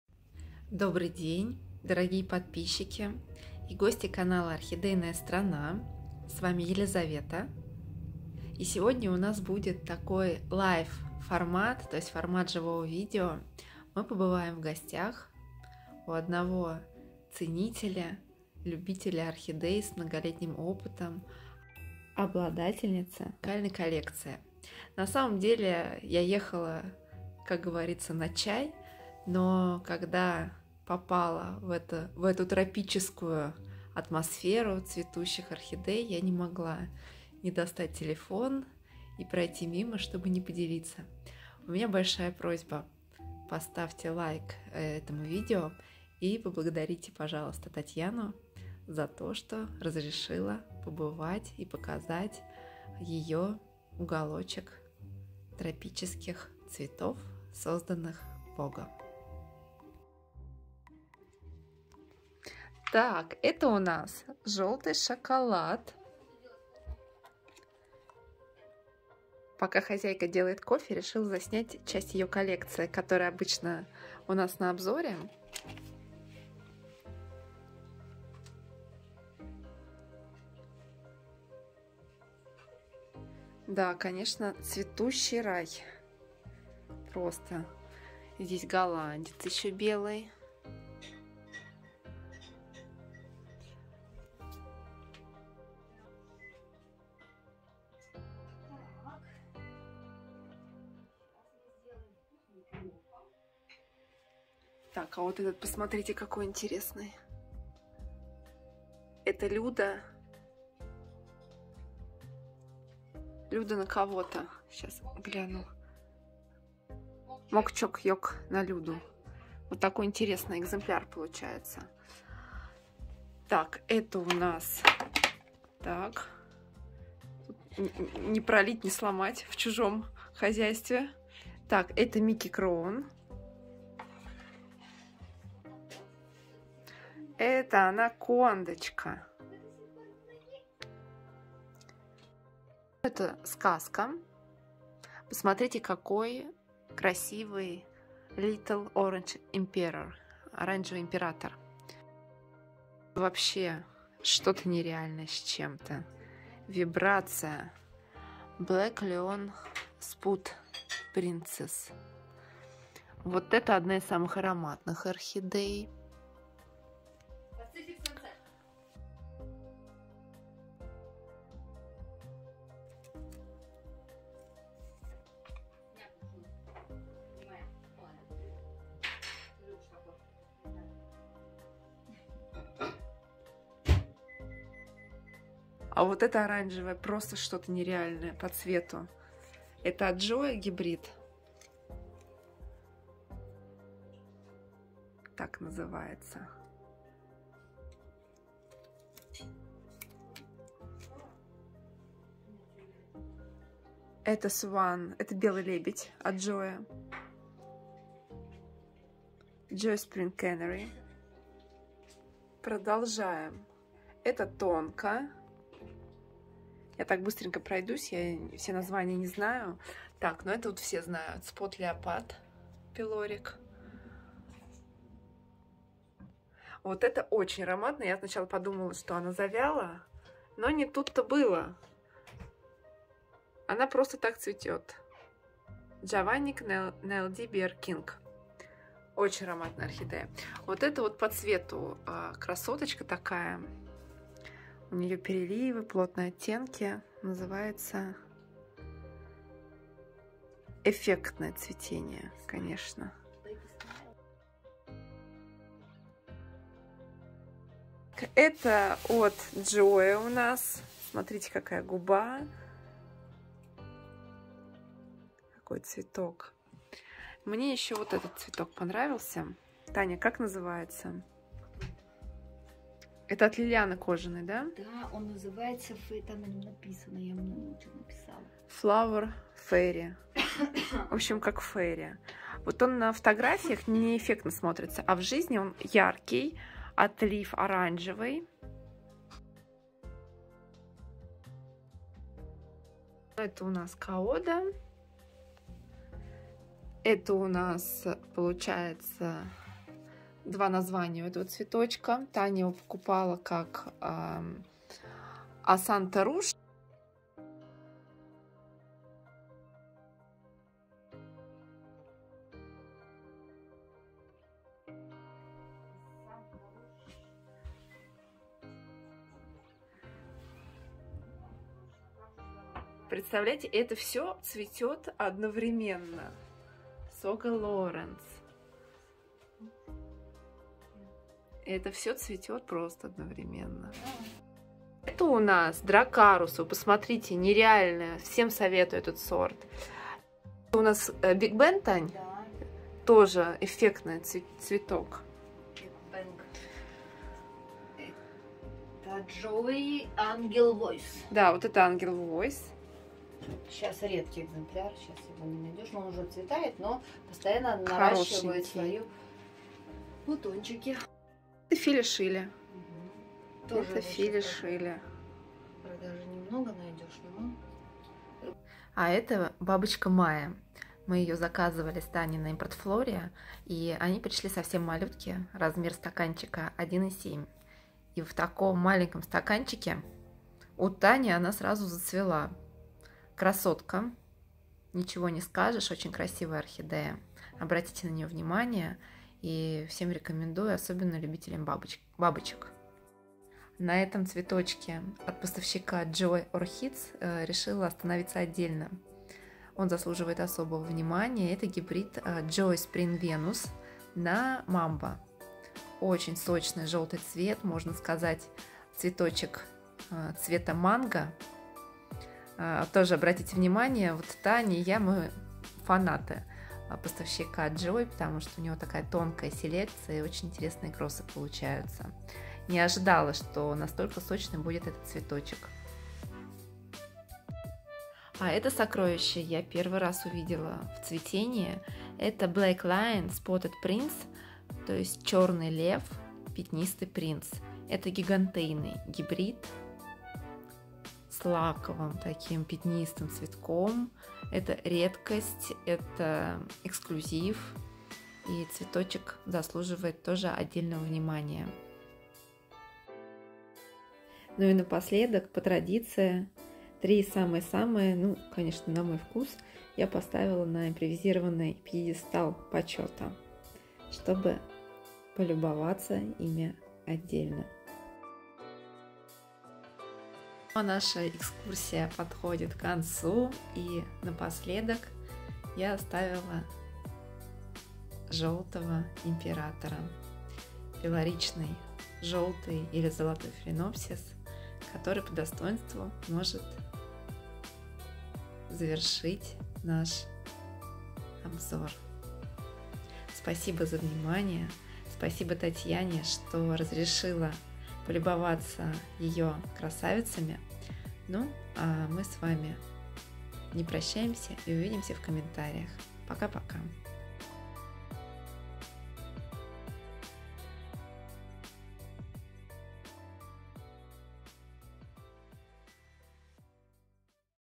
P. Black Lion 'Spotted Prince', philippinensis x schilleriana, Little Emperor peloric, Super Maya
1:05 Phal. Yellow Chocolate
1:12 Phal. Caribbean Sunset ES самая ароматная орхидея на данный момент цветущая в коллекции.
1:26 Phal. Ever Spring Prince
1:48 Phal. Mok Choi Yew x Lueddemanniana
2:05 Phal. Miki Crown
2:16 Phal. Zheng Min Anaconda
2:22 Phal. Joy Fairy Tale
2:25 7:59 Phal. Little Emperor peloric
4:02 Phal. Tying Shin Baby Smile
2:35 7:07 Phal. Black Lion 'Spotted Prince' (Phal. Paifang’s Golden Lion × Phal. Black Eagle)
2:45 Phal. Brother Sara Gold x KS Happy Eagle
3:25 Phal. Joy Snow Swan
3:29 Phal. Joy Spring Canary
3:33 Phal. Tonka
3:41 Phal. I-Hsin Spot Leopard 'peloric'
3:45
3:46 Phal. (javanica × LD's Bear King) × LD's Bear King 'RK6'
4:02 Phal. Tying Shin Baby Smile GW438
3:07 6:34 LD374 Phal. Joy Spring Venus x  Mambo
4:19 LD324 Phal. Rasa Veniana ( lueddemanniana x venosa) seedling
4:29 Phal. Flower Fary
4:54 Phal. Kaoda Twinkle
4:56 Phal. Asanta Rouge или Phal. Flame Rouge
5:15
5:21
5:40 Phal. I-Hsin The Big Bang
5:36 Phal. Dracarys
5:49 Phal. Joy Angel Voice
6:02 Phal. philippinensis x schilleriana
6:10 Phal. Super Maya 'peloric'